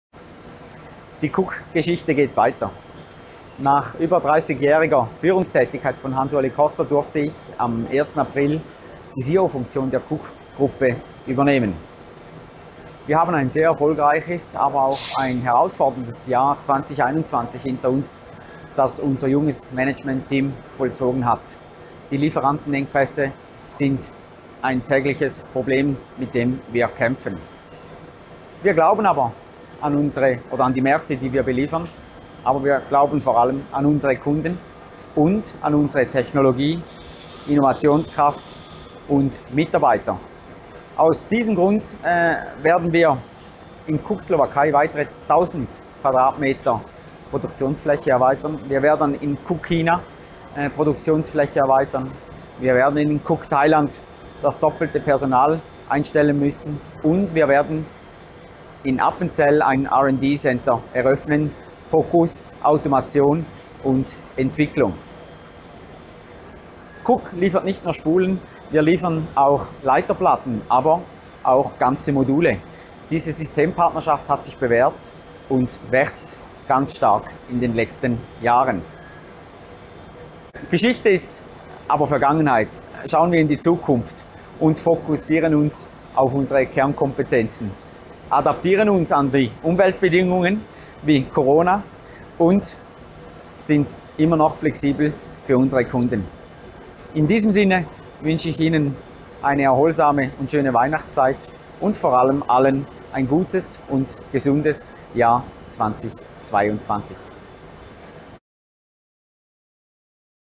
Wo steht die Elektrotechnikbranche? Gedanken zum Jahreswechsel 2021/22 von CEO Manuel Inauen
Im Jahr 2021 hat KUK Group nicht nur einen Generationenwechsel in der Unternehmensführung gemeistert, sondern auch viele Herausforderungen angesichts der angespannten Lieferketten. Natürlich musste auch KUK Kunden teils oft auf später vertrösten. Der hohen Nachfrage begnen wir mit einem Kapazitätsausbau an mehreren Standorten. Über die letzten Jahre hat sich KUK auch abseits von Spulen wickeln einen Namen in vielen Branchen gemacht: Die Montage kompletter Baugruppen sowie PCB Bestückung runden das Angebot ab.